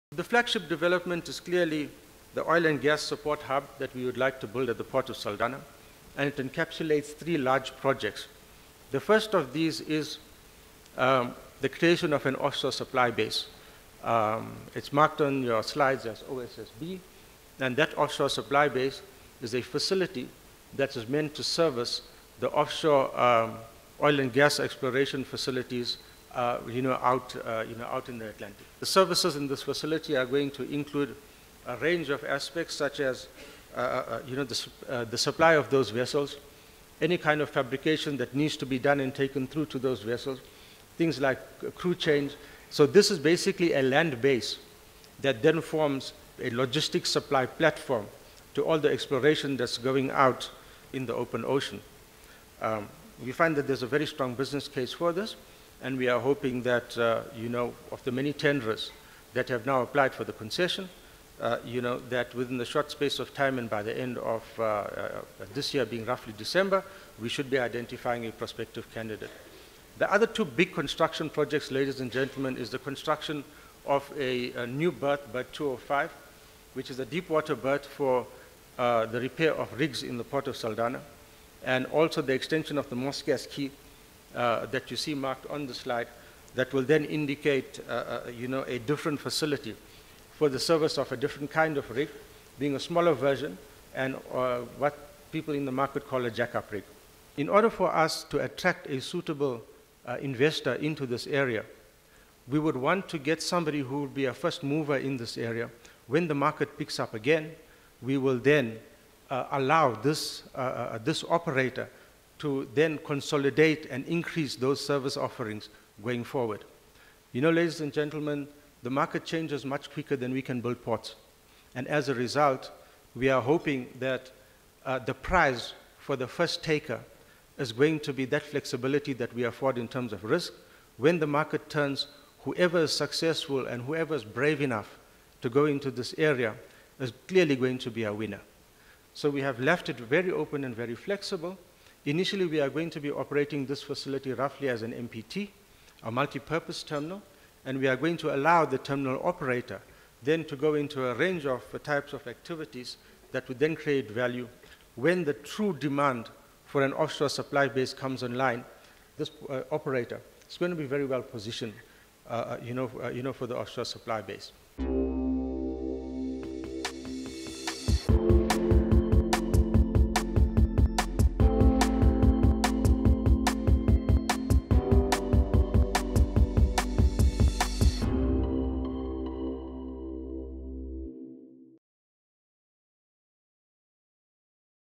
TNPA luring private oil-services investors to Saldanha using ‘flexible’ terminal concessions
Freight logistics group Transnet is optimistic of receiving positive responses to a tender for the development of an offshore supply base (OSSB) for the oil and gas industry in Saldanha Bay, reporting that around 120 people attended a tender site briefing this week.